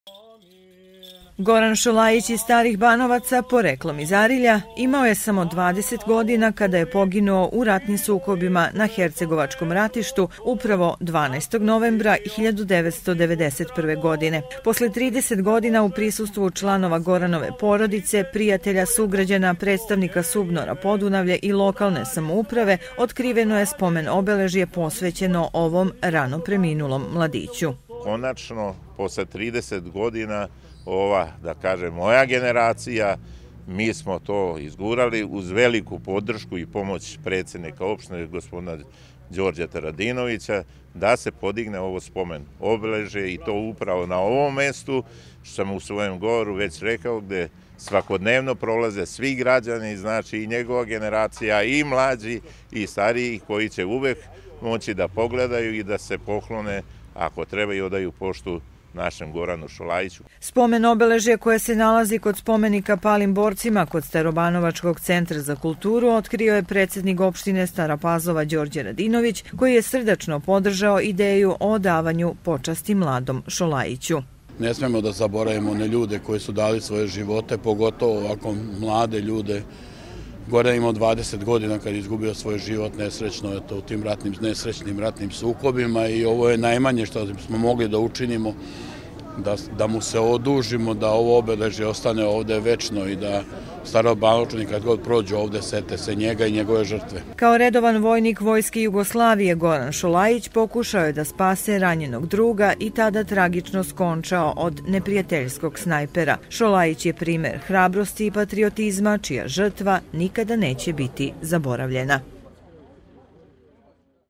Spomenik Goranu Šolajiću u Starim Banovcima
Danas je u Starim Banovcima kod spomenika palim borcima otkriven spomenik Goranu Šolajiću, mladiću koji je 1991.godine poginuo u ratnim sukobima.